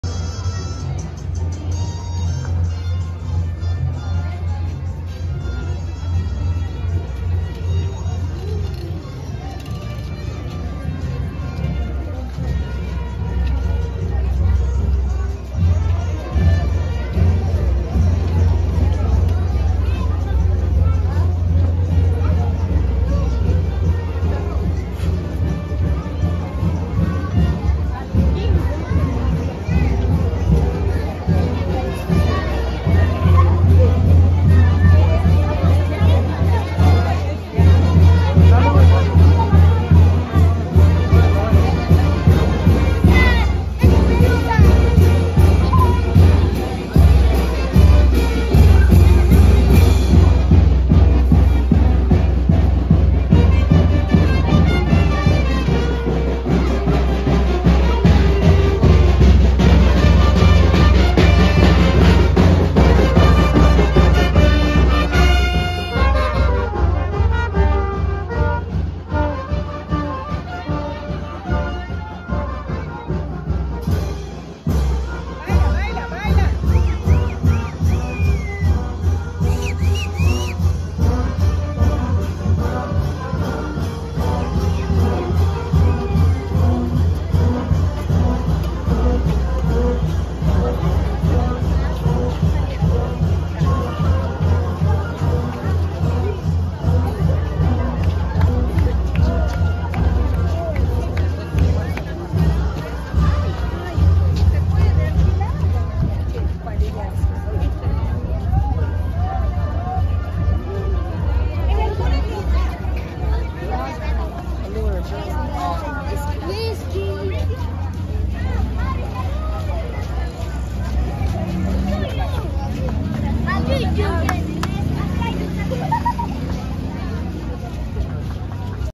CON PASACALLE CELEBRAN DÍA DE LA CANCIÓN CRIOLLA EN HUARI
Por conmemorarse el Día de la Canción Criolla y previas al Día de Todos los Santos, los docentes, niños y niñas de la Institución Educativa Parroquial Integrado "Silvia Ruff", realizaron el colorido pasacalle por las diferentes calles de la ciudad de Huari.